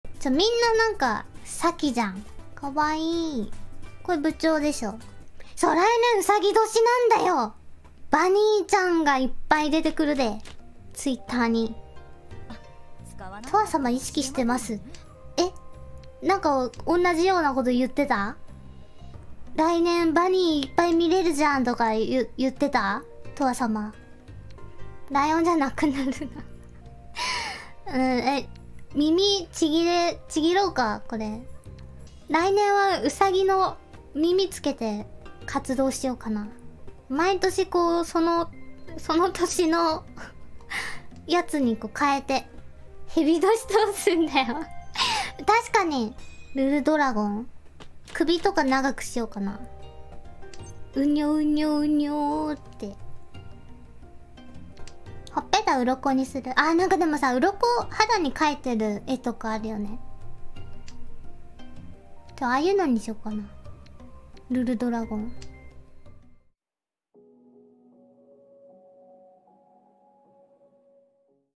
Rurudo In Bunny Outfit? (Rurudo) [Eng Subs]
Rurudo ponders becoming a bunny to fit in with 2023 being the Year of the Rabbit...

● Source:
麻雀――――――。